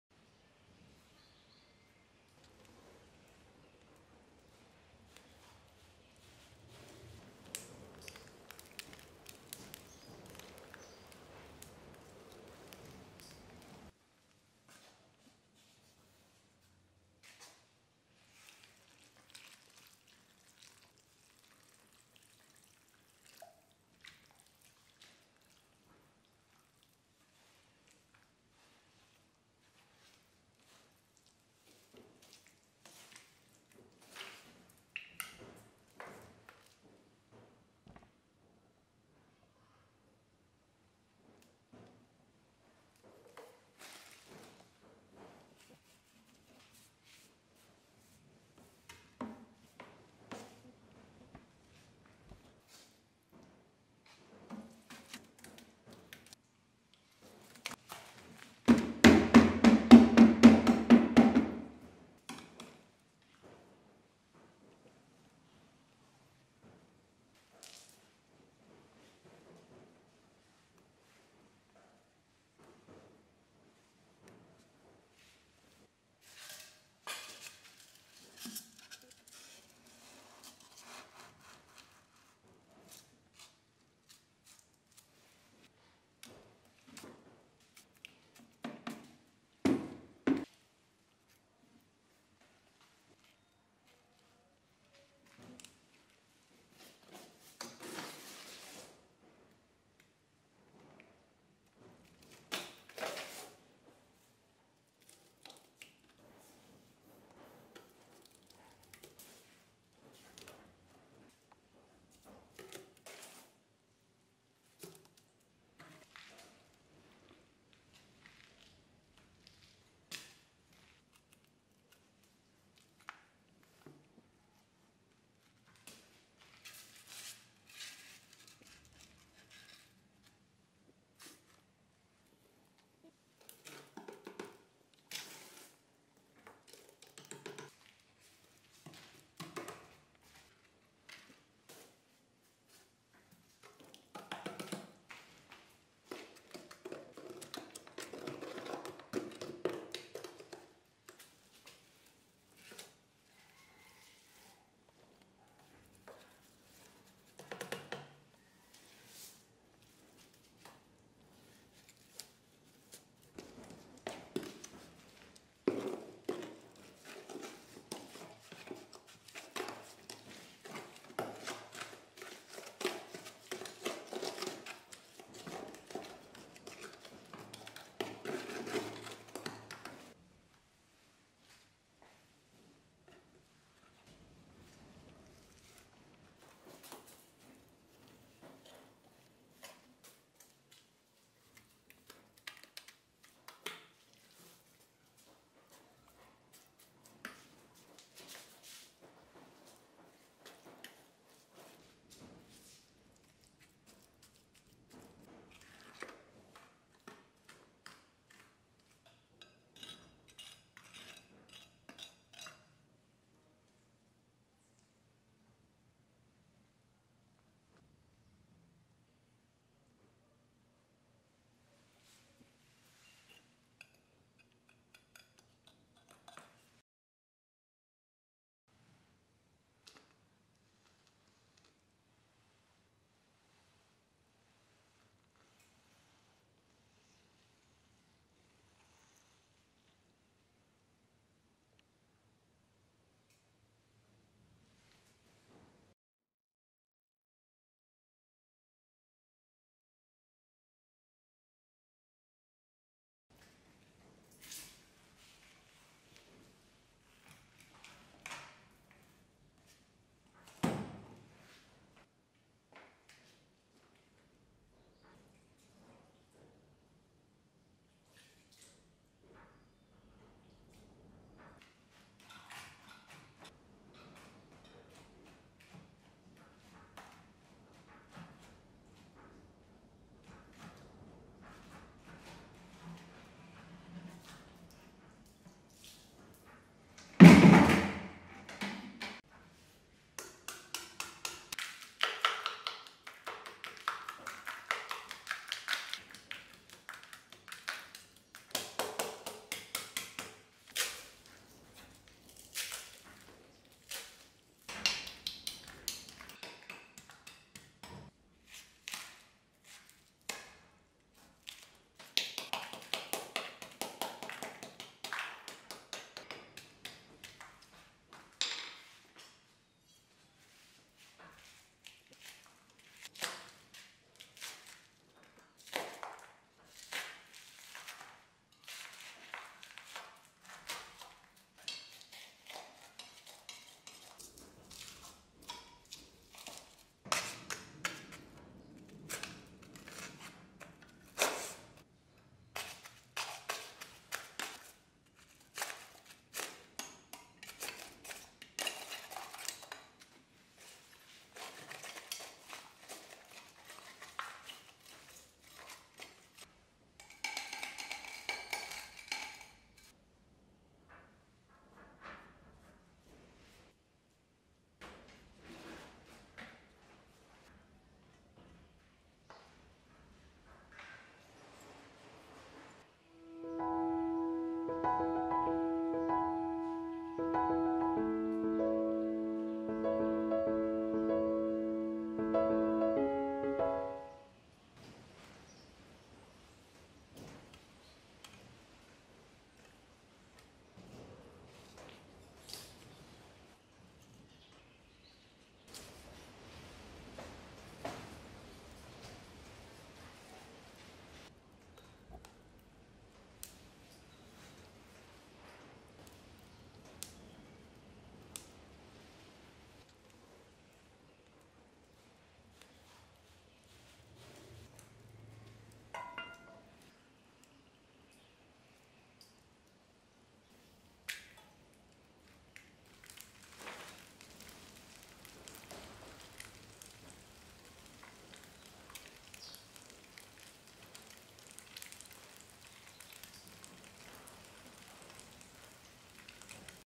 How to cast a wood stove with an old bucket
Making a wood stove from an old bucket (ASMR)